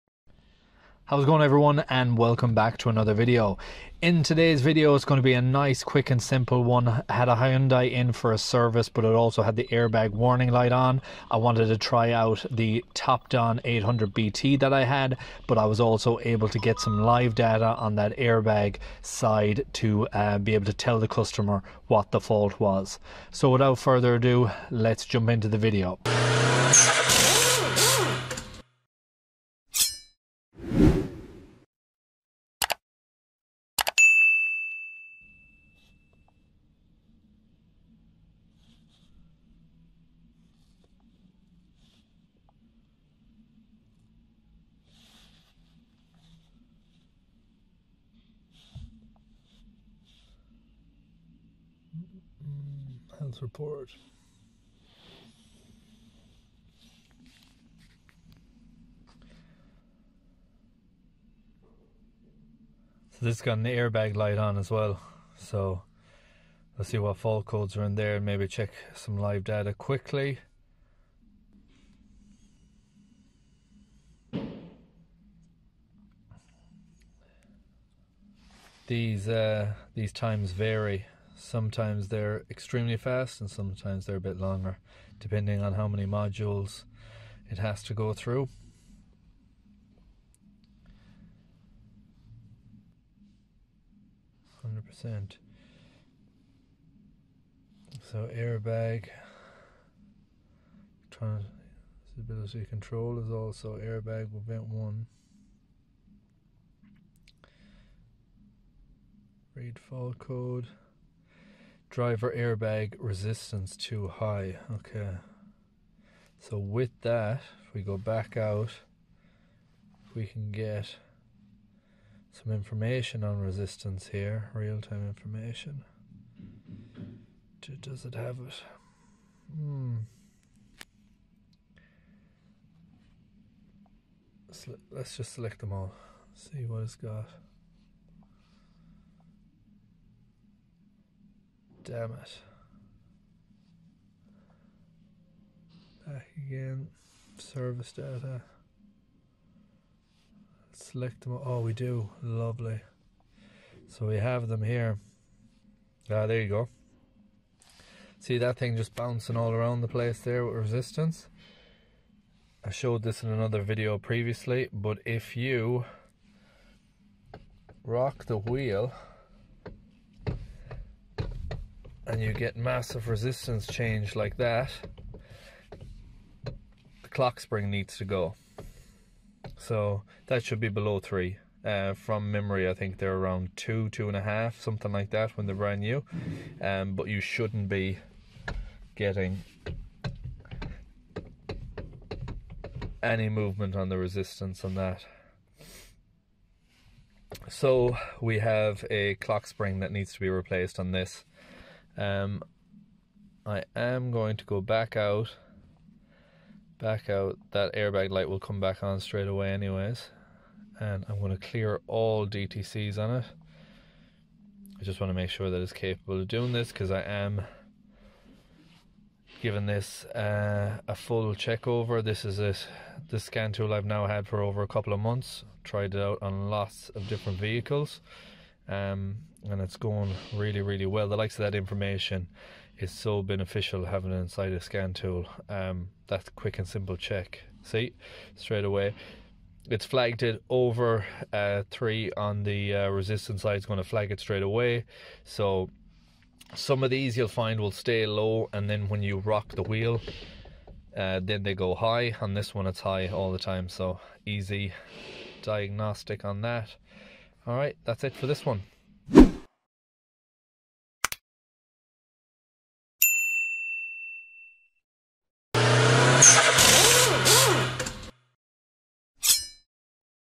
Hyundai - Kia Airbag Fault - Diagnosing Tip Using Topdon ArtiDiag800BT
In this video I show how to diagnose an Airbag fault on a Hyundai or Kia, I am currently trialing the TopDon ArtiDiag 800BT and thought this video would be useful in demonstrating the capabilities of using live data on this DIY to mid range scan tools.

Currently on promo until 14th August 2022 - I have been given a discount link for subscribers to use here ➢
(I do not get paid anything from the sales of these scan tools)

If I helped ya out and you would like to buy me a drink

Thanks to everyone who watches my videos, Likes, Shares and or leaves a comment. I appreciate everyone of you.

Hyundai and Kia's have common failures with the airbag clock spring
Here are some related codes that a faulty clock spring can store
B1346/B134600 Driver Airbag Resistance too high (1st stage)
 B1347/B134700 Driver Airbag Resistance too low (1st stage)
 B1481/B148100 Driver Airbag Resistance too high (2nd stage)
 B1482/B148200 Driver Airbag Resistance too low (2nd stage)
 B1348/B134800 Driver Airbag resistance circuit short to Ground (1st stage)
 B1349/B134900 Driver Airbag resistance circuit short to Battery (1st stage)
 B1483/B148300 Driver Airbag resistance circuit short to Ground (2nd stage)
 B1484/B148400 Driver Airbag resistance circuit short to Battery (2nd stage)